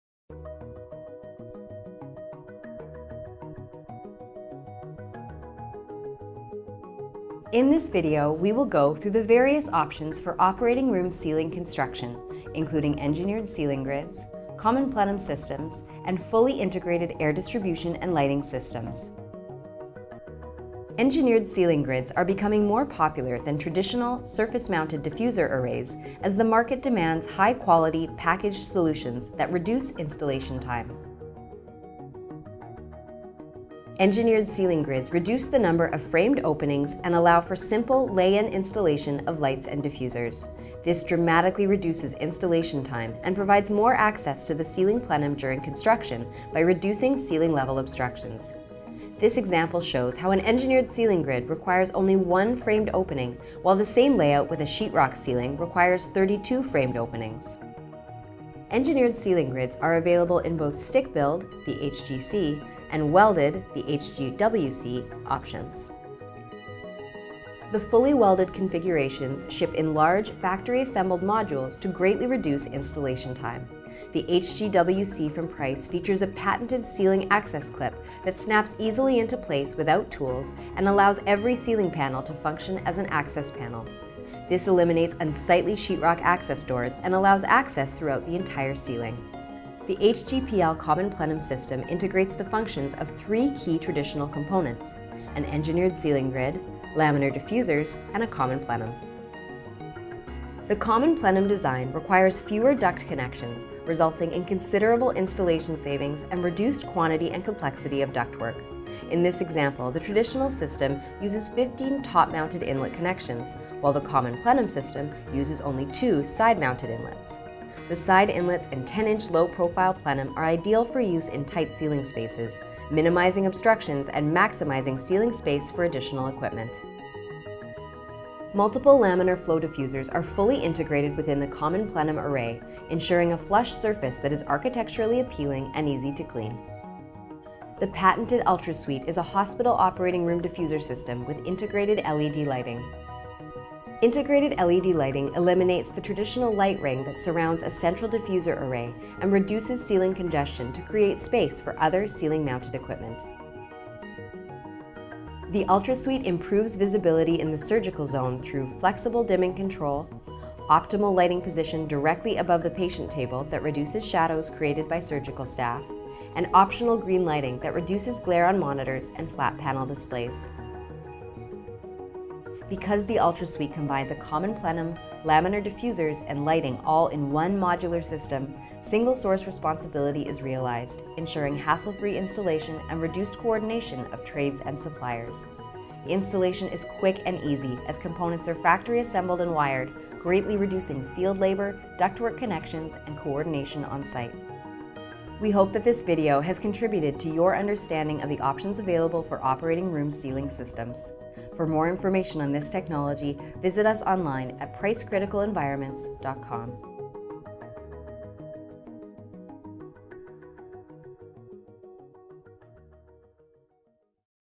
Operating Room Ceiling Systems
Discover the construction options available for operating room ceiling systems.